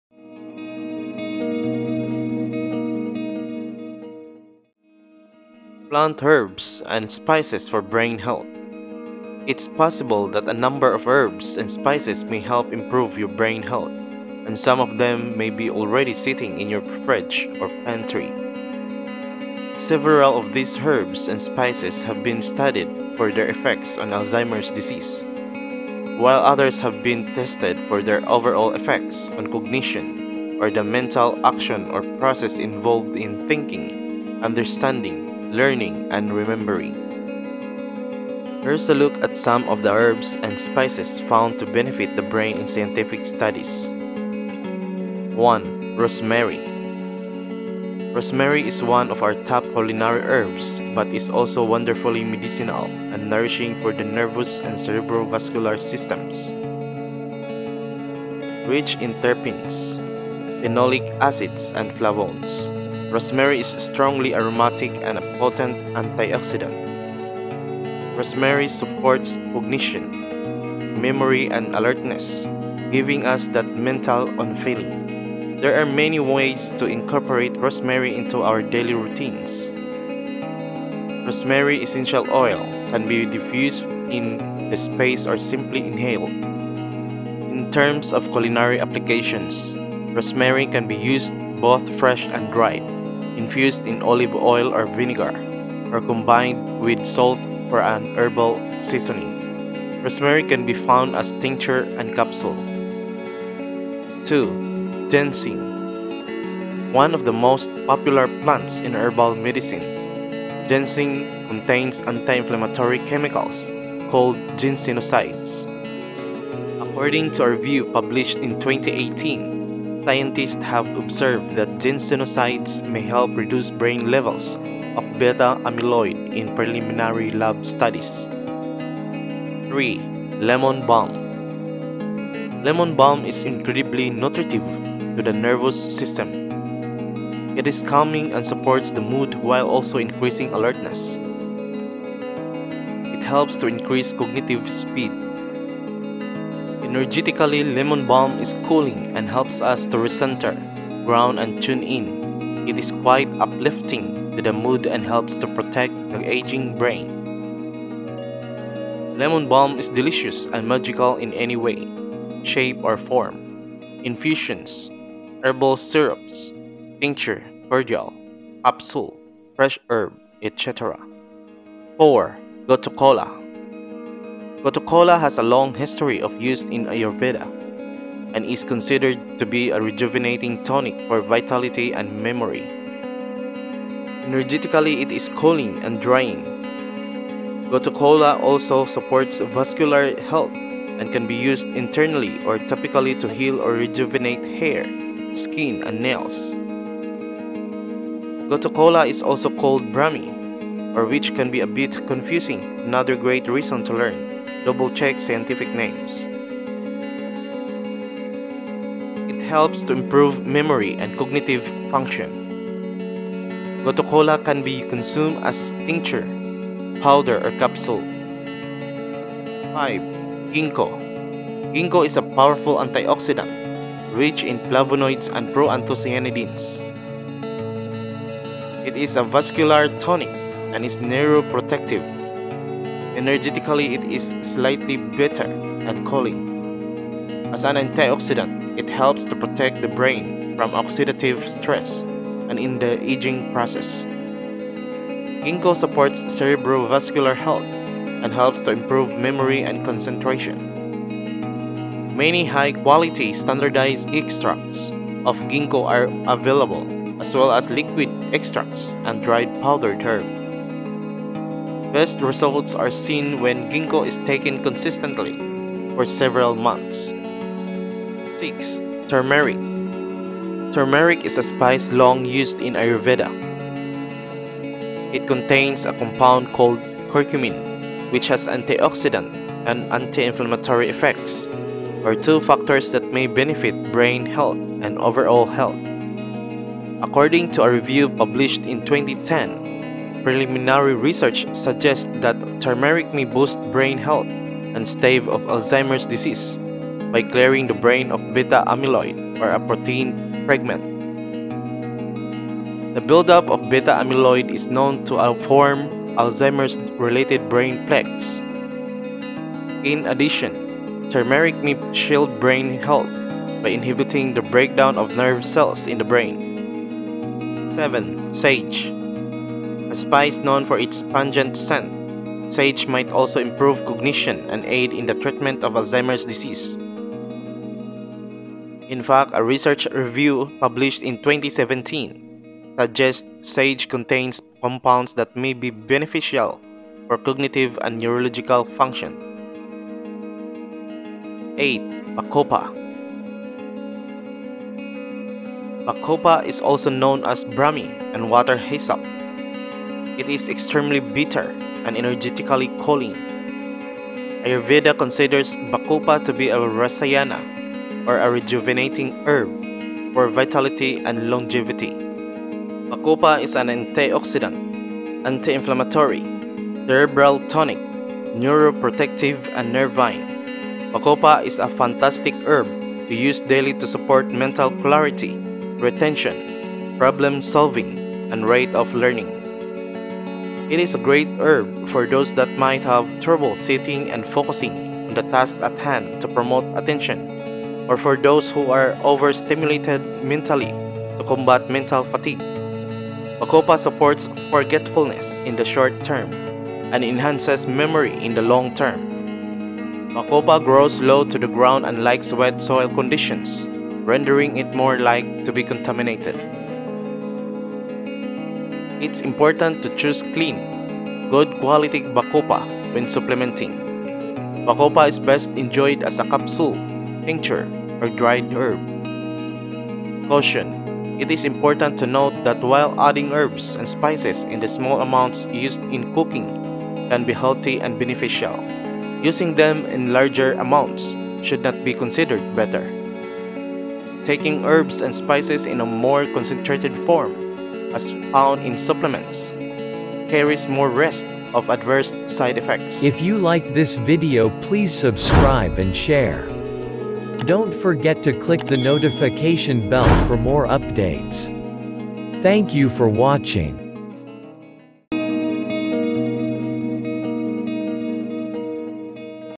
PLANT HERBS AND SPICES FOR BRAIN HEALTH| 8 PLANTS FOR BRAIN ENHANCER
Plant Herbs and Spices for Brain Health.
1. Rosemary.
Rosemary is one of our top culinary herbs, but is also wonderfully medicinal and nourishing for the nervous and cerebrovascular systems. Rich in terpenes, phenolic acids and flavones, rosemary is strongly aromatic and a potent antioxidant. Rosemary supports cognition, memory and alertness, giving us that mental “on”feeling. There are many ways to incorporate rosemary into our daily routines. Rosemary essential oil can be diffused in the space or simply inhaled. In terms of culinary applications, rosemary can be used both fresh and dried, infused in olive oil or vinegar, or combined with salt for an herbal seasoning. Rosemary can be found as a tincture and capsule.
2. Ginseng.
One of the most popular plants in herbal medicine, ginseng contains anti-inflammatory chemicals called ginsenosides. According to a review published in 2018, scientists have observed that ginsenosides may help reduce brain levels of beta-amyloid in preliminary lab studies.
3. Lemon Balm.
Lemon balm is incredibly nutritive to the nervous system. It is calming and supports the mood while also increasing alertness. It helps to increase cognitive speed. Energetically lemon balm is cooling and helps us to recenter, ground and tune in. It is quite uplifting to the mood and helps to protect the aging brain. Lemon balm is delicious and magical in any way, shape or form — infusions, herbal syrups, tincture, cordial, capsule, fresh herb, etc.
4. Gotu Kola.
Gotu kola has a long history of use in Ayurveda, and is considered to be a rejuvenating tonic for vitality and memory. Energetically it is cooling and drying. Gotu kola also supports vascular health and can be used internally or topically to heal or rejuvenate hair, skin and nails. Gotu kola is also called brahmi (which can be a bit confusing! another great reason to learn/double check scientific names). It helps to improve memory and cognitive function. Gota kola can be consumed as a tincture, powder or capsule.
5. Gingko.
Gingko is a powerful antioxidant, rich in flavonoids and proanthocyanidins. It is a vascular tonic and is neuroprotective. Energetically it is slightly bitter and cooling. As an antioxidant, it helps to protect the brain from oxidative stress and in the aging process. Gingko supports cerebrovascular health and helps to improve memory and concentration. Many high quality standardized extracts of gingko are available, as well as liquid extracts and dried powdered herb. Best results are seen when gingko is taken consistently for several months.
6. Turmeric.
Turmeric is a spice long used in Ayurveda. It contains a compound called curcumin, which has antioxidant and anti-inflammatory effects (two factors that may benefit brain health and overall health).
According to a review published in 2010, preliminary research suggests that turmeric may boost brain health and stave off Alzheimer's disease by clearing the brain of beta-amyloid (a protein fragment). The buildup of beta-amyloid is known to form Alzheimer's-related brain plaques.
In addition, turmeric may shield brain health by inhibiting the breakdown of nerve cells in the brain.
7. Sage.
A spice known for its pungent scent, sage might also improve cognition and aid in the treatment of Alzheimer's disease. In fact, a research review published in 2017, suggests sage contains compounds that may be beneficial for cognitive and neurological function.
8. Bacopa (Bacopa monnieri)
Bacopa is also known as Brahmi and water hyssop. It is extremely bitter and energetically cooling. Ayurveda considers bacopa to be a rasayana, or a rejuvenating herb for vitality and longevity. Bacopa is an antioxidant, anti-inflammatory, cerebral tonic, neuroprotective and nervine. Bacopa is a fantastic herb to use daily to support mental clarity, retention, problem solving and rate of learning. It is a great herb for those that might have trouble sitting and focusing on the task at hand to promote attention, or for those who are over-stimulated mentally to combat mental fatigue. Bacopa supports forgetfulness in the short term, and enhances memory in the long term. Bacopa grows low to the ground and likes wet soil conditions, rendering it more likely to be contaminated. It’s important to choose clean, good quality bacopa when supplementing. Bacopa is best enjoyed as a capsule, tincture or dried herb.